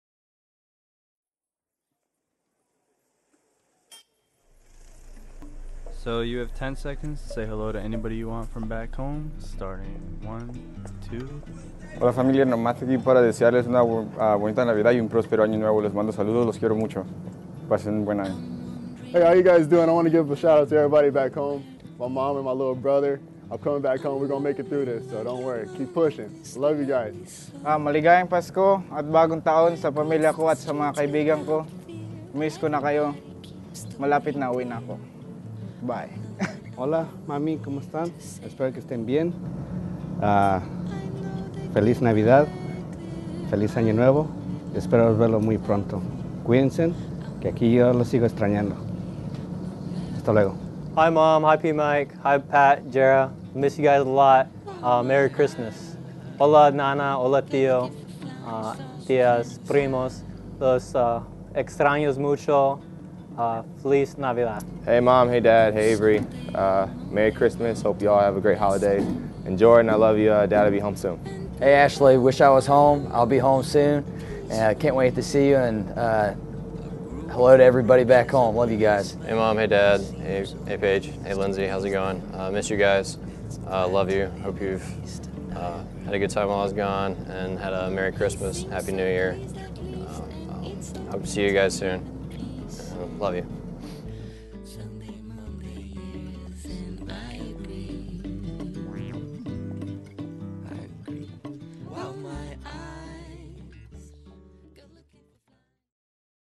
USS Harpers Ferry Holiday Greetings - Engineering and Electrical Department
The Engineering and Electrical Departments aboard USS Harpers Ferry (LSD 49) say hello this Holiday Season.